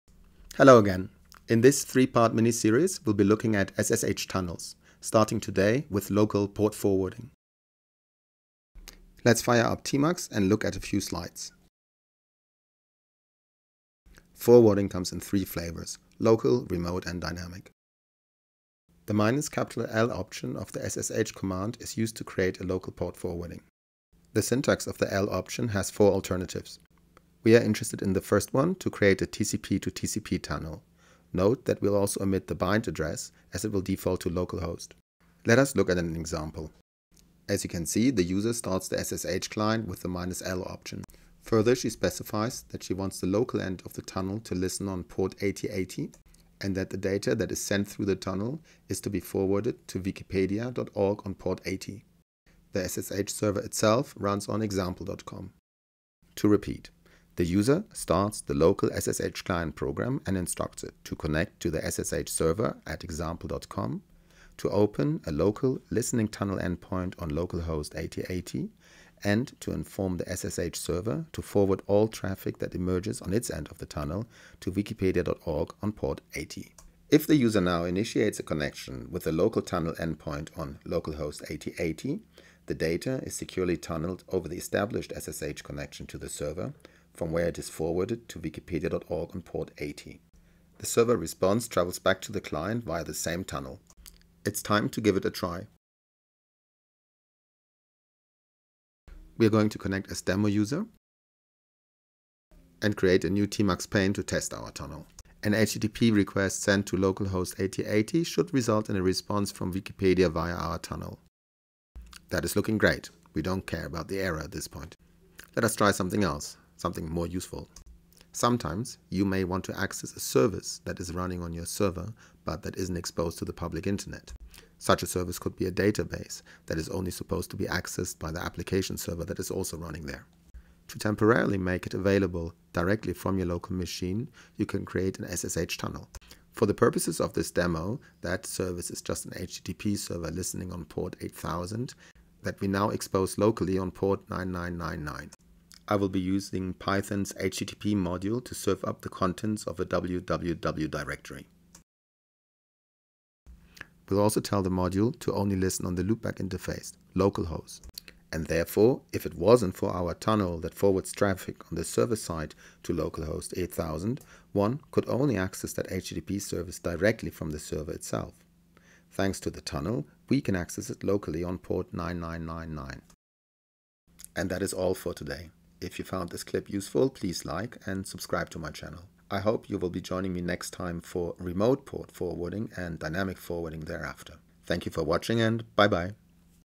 SSH tunnel for local port forwarding
Learn how to create SSH tunnels to forward traffic arriving at local TCP ports to remote destinations over a secure channel.

The next installment of this mini-series will be looking at Remote port forwarding and subsequently at Dynamic forwarding via SOCKS proxies.